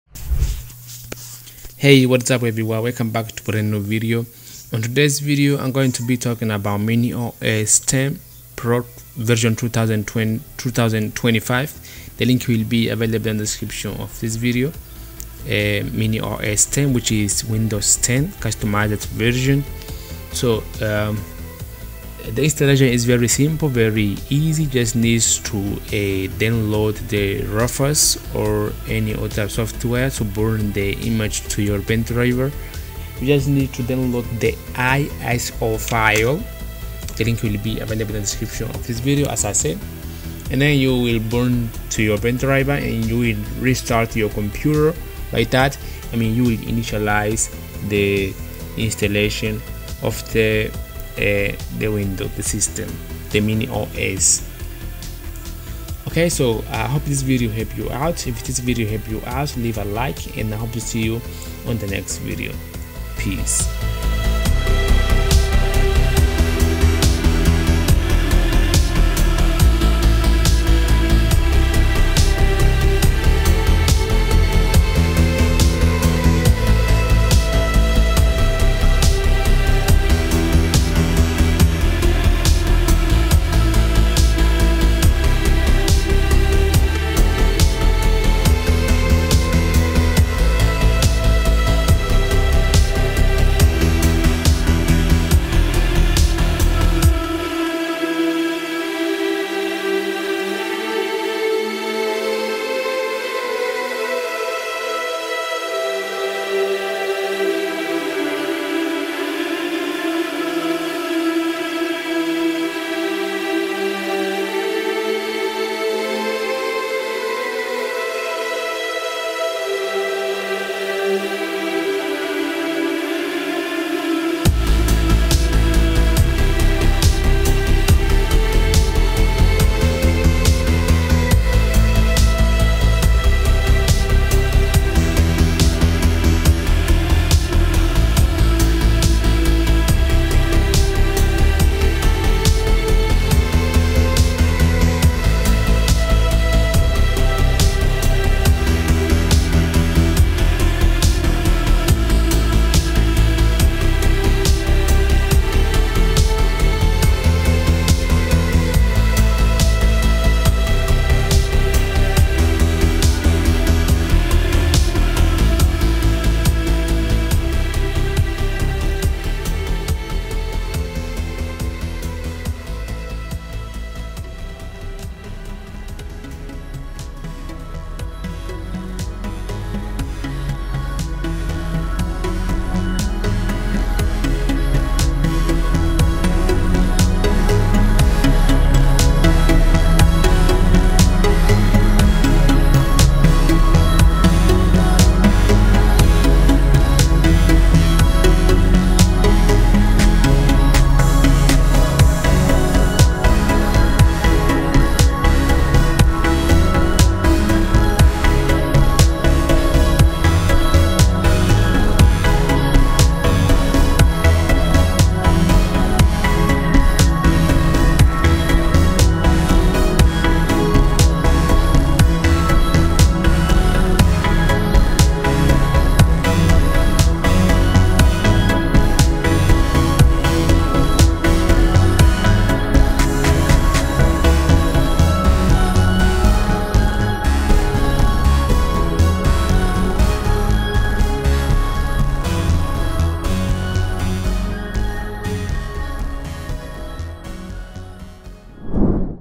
MiniOS 10 - Fastest Windows 10 Mod?
In this video, I take a deep dive into MiniOS 10, a lightweight and performance-optimized version of Windows 10 based on the 22H2 update. Is this the fastest Windows 10 mod out there? Let's find out!

What's inside the video:

— Background process usage 🔄
— RAM and memory consumption 💾
— Pre-installed software and key features ⚙️
— Update status and system performance 🚀
— Windows version info (22H2 build) 🧩

Whether you're a gamer, student, or just want a clean and fast Windows experience for your low-end PC, this might be what you're looking for.